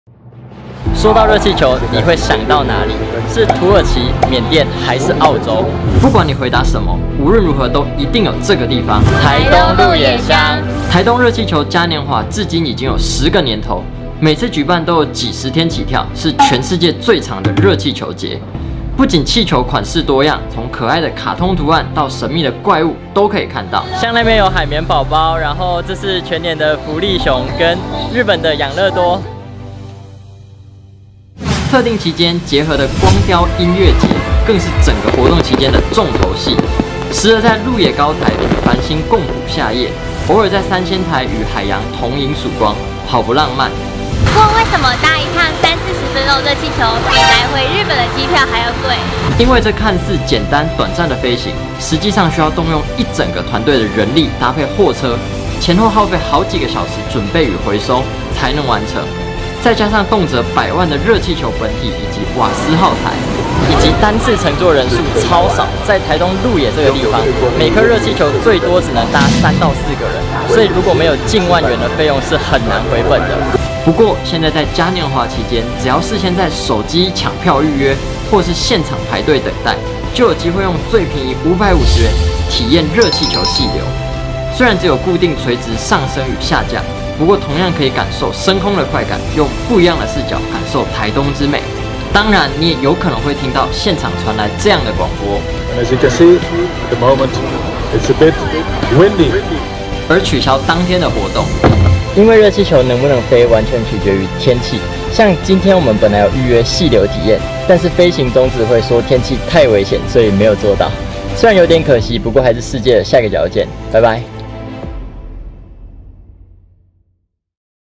【小鎮漫遊】鹿野高台 國際熱氣球嘉年華！搭熱氣球到底在貴什麼？參加活動全攻略：台東鹿野鄉｜台灣368鄉鎮系列 EP. 3
搭一次熱氣球比來回日本機票還貴！
到底為什麼？